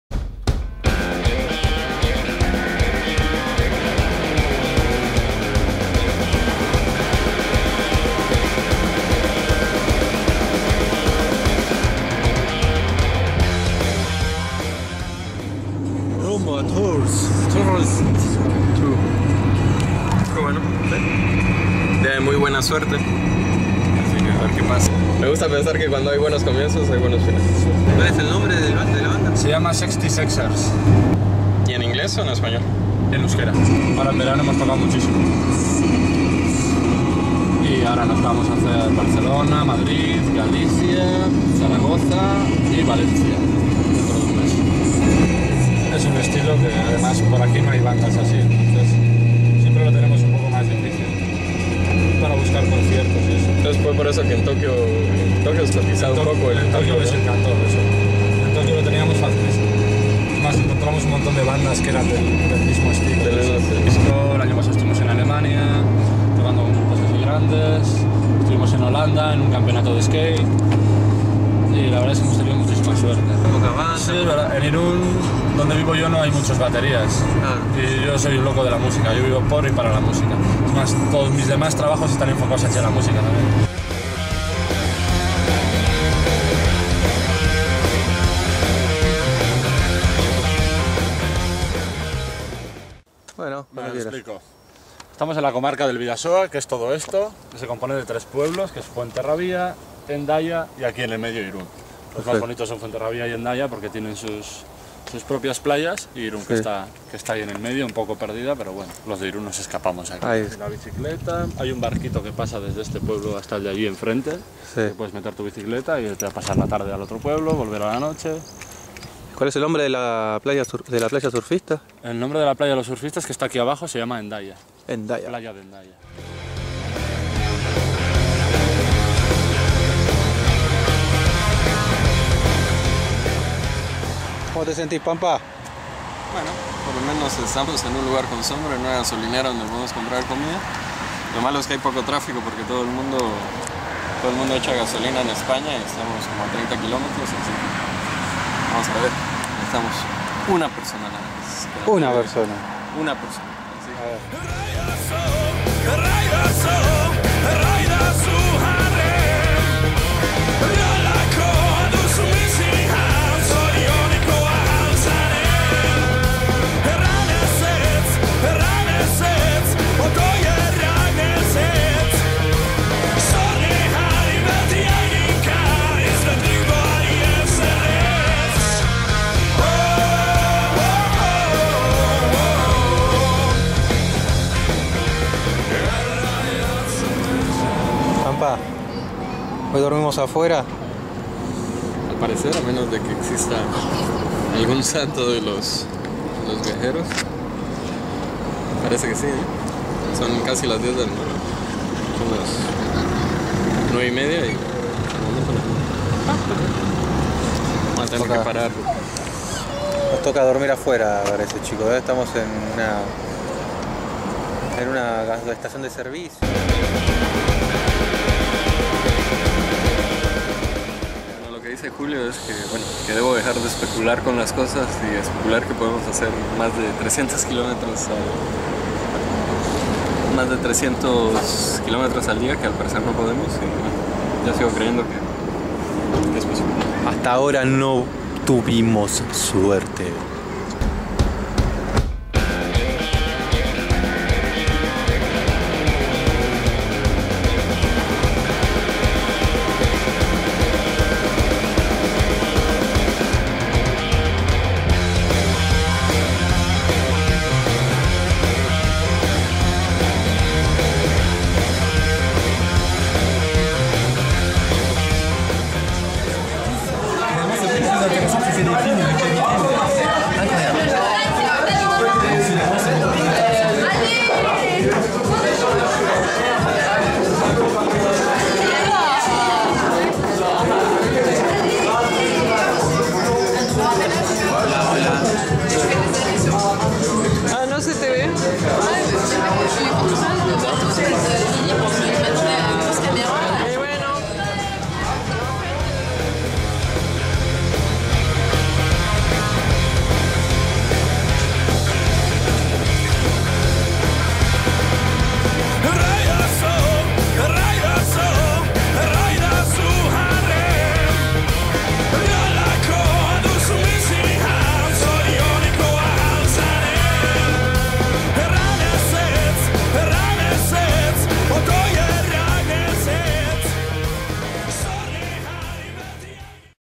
Tours diario mochilero episodio 11 el camino desde Mondragon
Nos vamos de Mondragón y directamente a encarar la ruta para intentar llegar a Tours Francia... pero nos tocó dormir afuera!